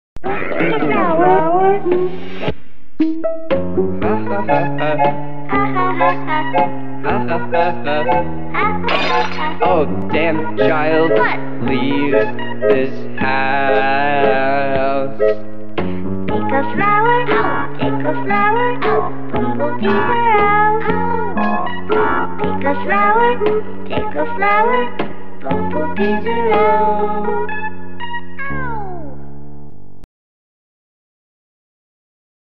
Pick a flower big collab (cerrado)
Reglas(importante seguir!!)

Te dejó una semana para hacer tu parte

2 partes por persona

Puedes poner audio

Colores que debes usar: los colores de tu oc o Fondo negro y lineart blanco

Saber que es un big collab

Respetar las partes ocupadas es Désir no empezar a amenazar o insultar si no tienes la/s parte/s que querias

Poner tu nombre obligatorio

No intros ni outros

Se aceptan marcas de agua